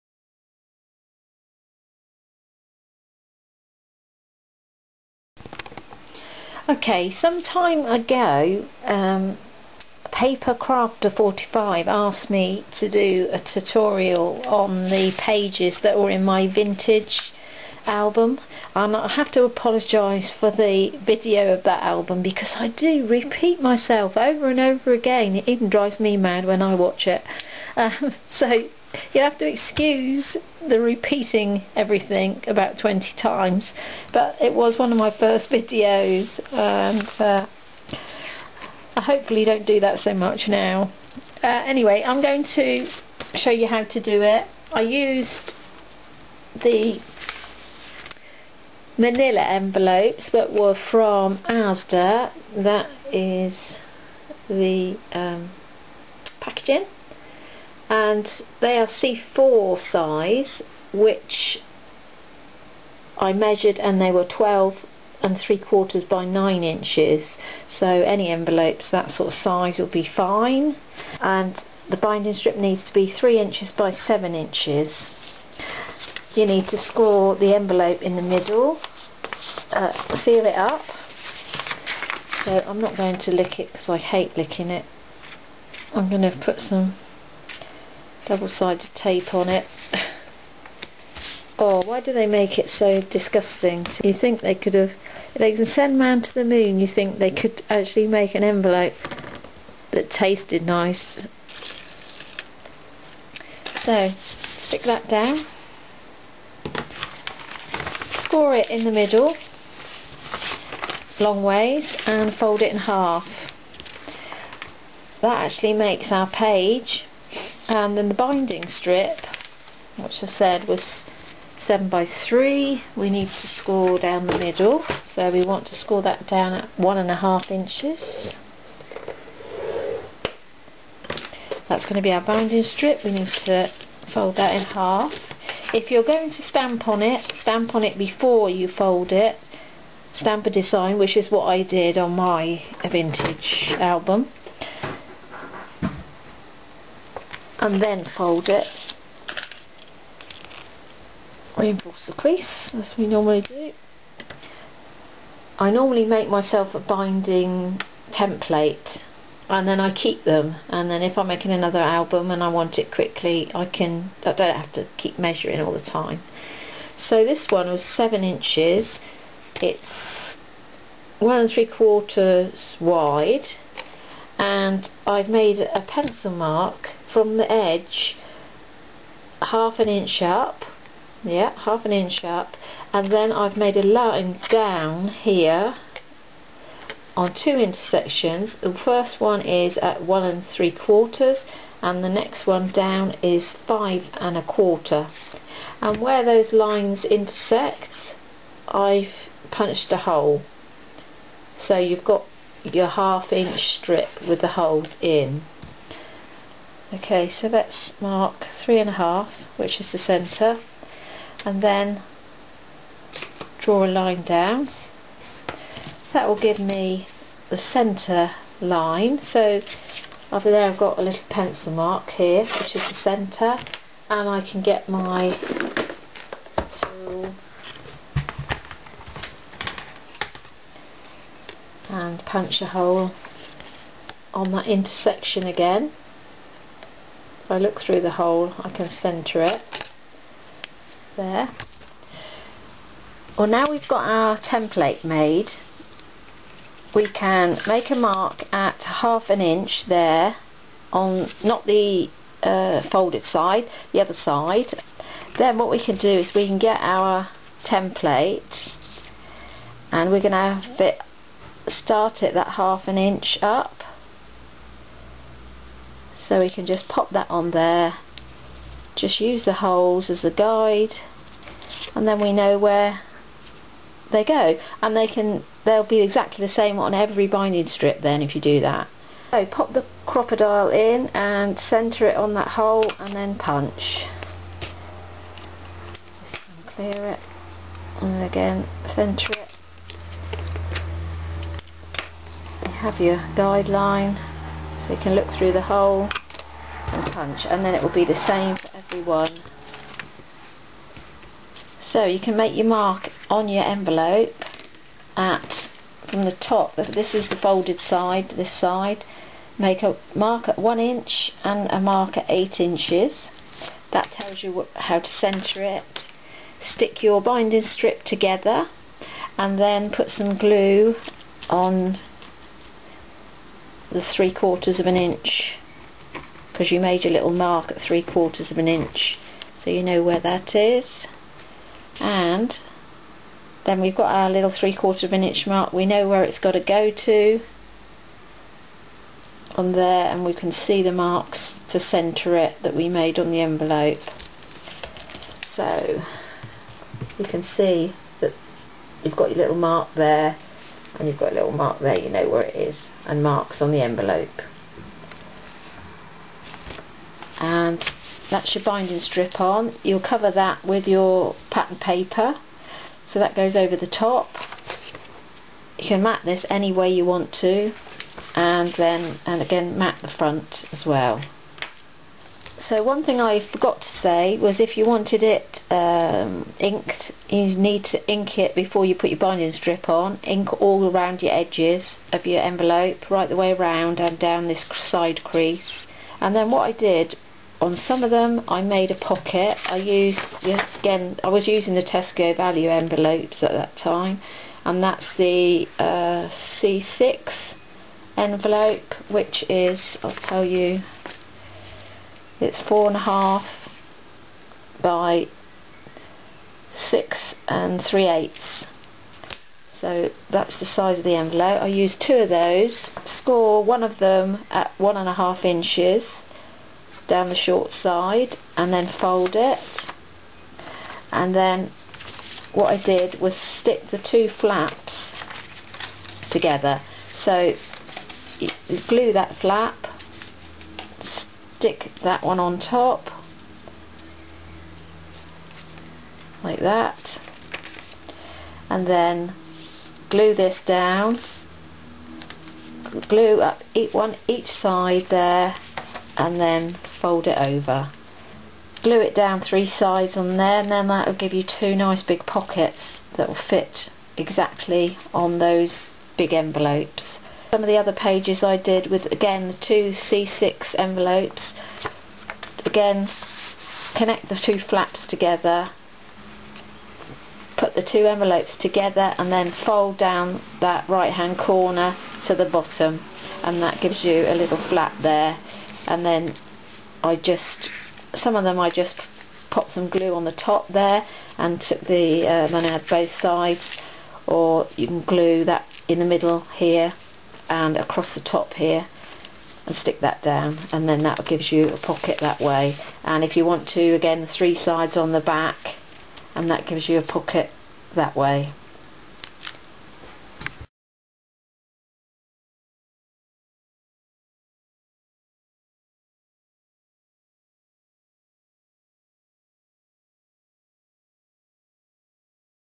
How to make some of the vintage photo album pages
paper crafter 45 asked me quiet a while ago about doing a tutorial on the vintage photo album pages.  Sorry its taken so long but I've uploaded it now.
You can look at the video for the album to see the decorated pages and pockets, but you will have to excuse me repeating myself a million times as this was one of my first videos, hopefully I'm a little better now!!!